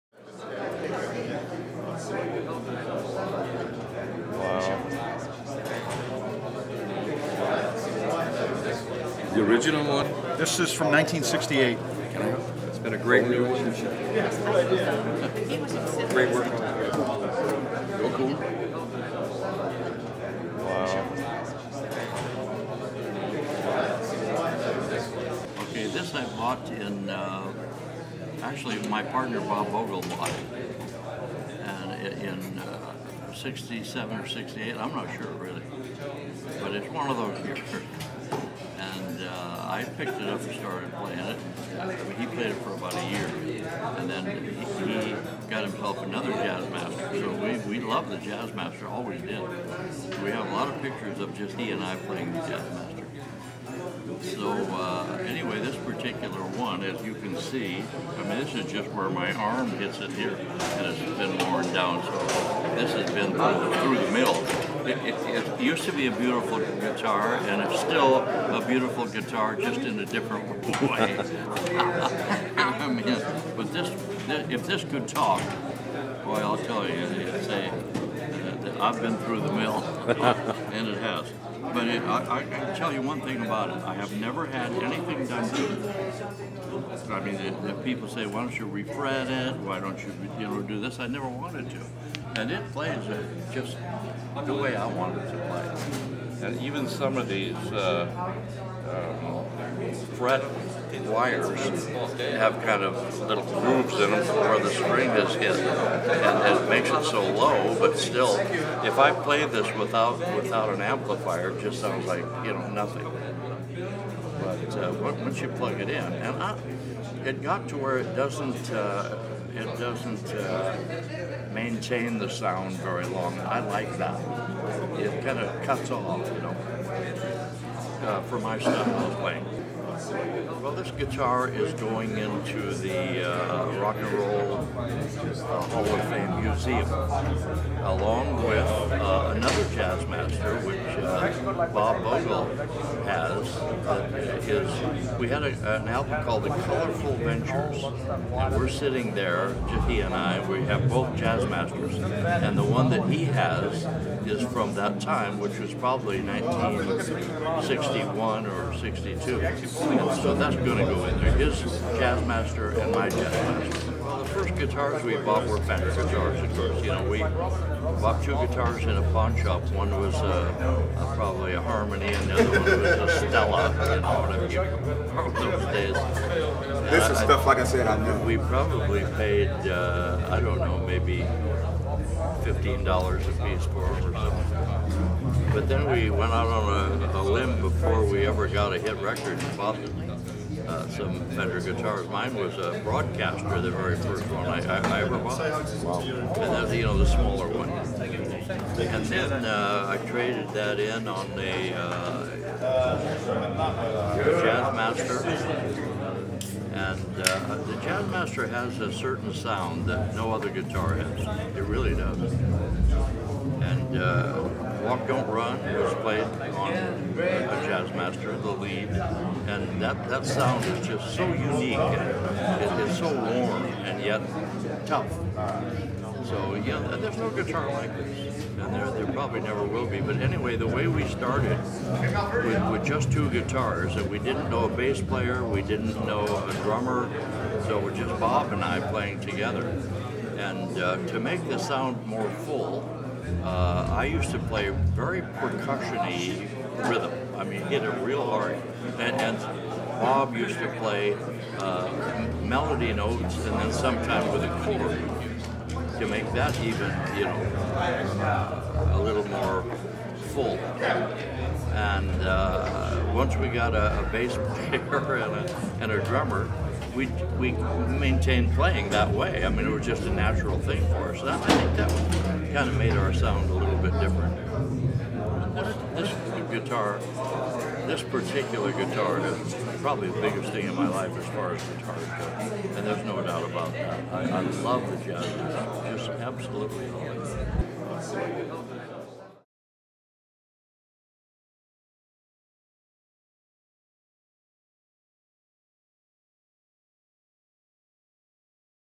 Don Wilson talks about his Fender® Jazzmaster® guitar
Guitarist Don Wilson of famed surf instrumental group the Ventures talks about his Fender Jazzmaster® guitar.